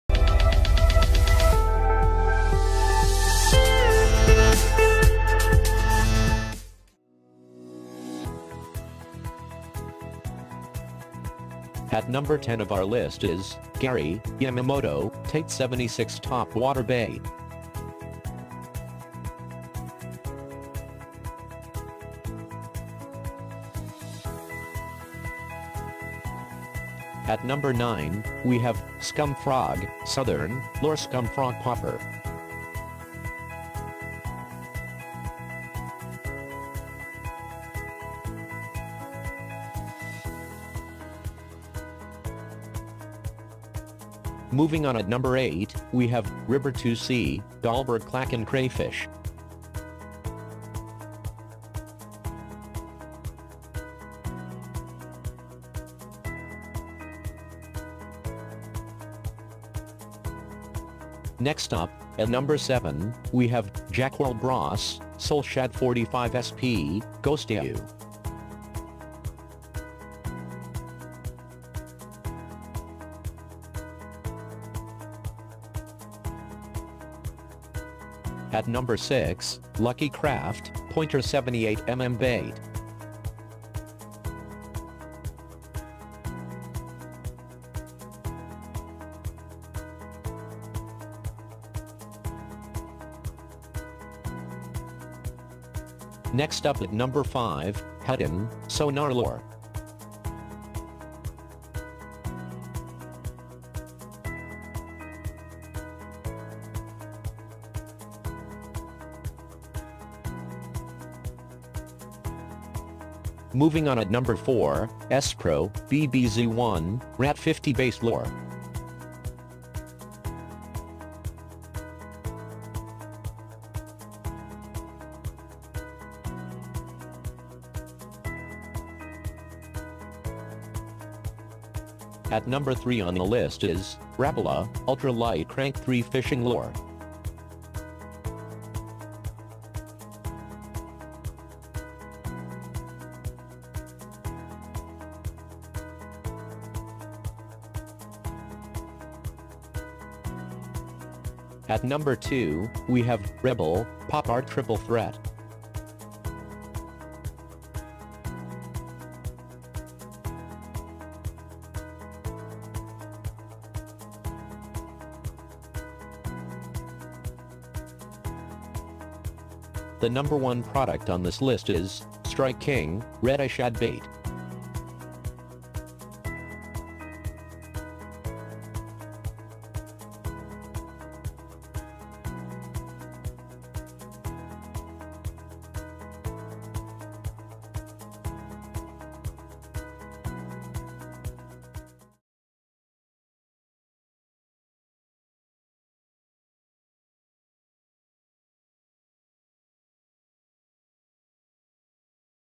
10 Best Bass Lures I Top Bass Lures 2018
10 Best Bass Lures I Top Bass Lures 2018

In this video, 10 Best Bass Lures. This 10 best lists made based on our personal opinion and try to list them based on the price quality durability and more. if you want to find out the price and more information about these, you can simply check out the link in the description above.